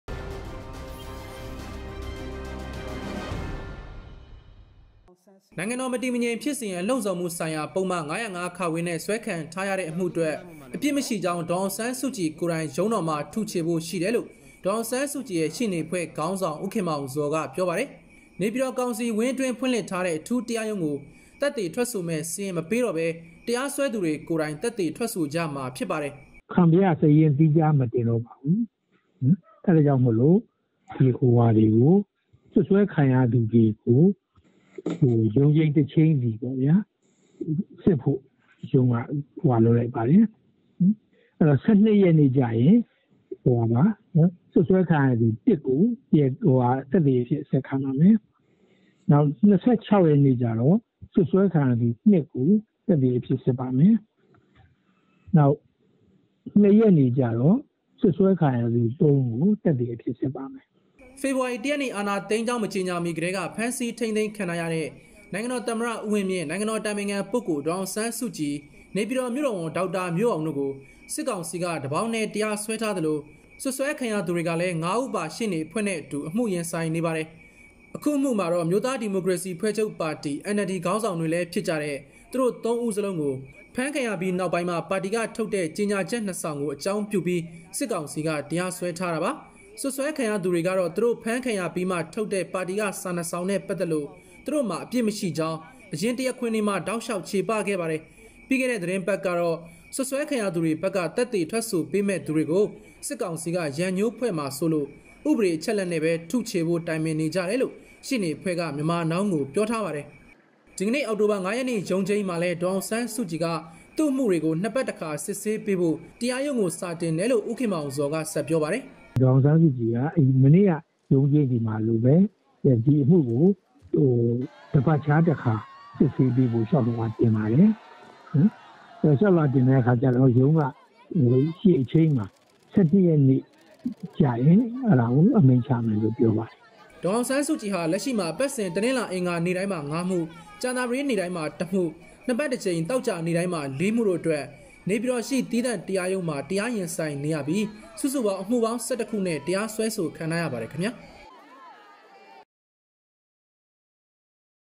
၅၀၅ အမှုတွင် လူသက်သေမတင်ဘဲ ဒေါ်အောင်ဆန်းစုကြည် ကိုယ်တိုင်ထုချေမည်
နိုင်ငံတော်မတည်မငြိမ်ဖြစ်စေရန် လှုံ့ဆော်မှုဆိုင်ရာ ပုဒ်မ ၅၀၅(ခ) အမှုတွင် အပြစ်မရှိကြောင်း သက်သေထွက်ပေးမည့် လူစာရင်းကို နေပြည်တော်ကောင်စီဝင်းအတွင်း သီးသန့်ဖွင့်လှစ် စစ်ဆေးနေသည့် တရားရုံးသို့  မတင်တော့ဘဲ ဒေါ်အောင်ဆန်းစုကြည်ကိုယ်တိုင်ထုချေရန် ရှိသည်။

ပြီးခဲ့သည့် ရက်သတ္တပတ်အတွင်း Myanmar Now က မေးမြန်းစဉ်ကမူ သက်သေထွက်မည့်သူကို စစ်ကောင်စီက ရန်ငြိုးဖွဲ့မည်စိုး၍ ဥပဒေအကိုးအကား အချက်အလက်ဖြင့်သာ ထုချေရန် စွပ်စွဲခံရသူများနှင့် တိုင်ပင်နေကြောင်း ဦးခင်မောင်ဇော်က ပြောထားသည်။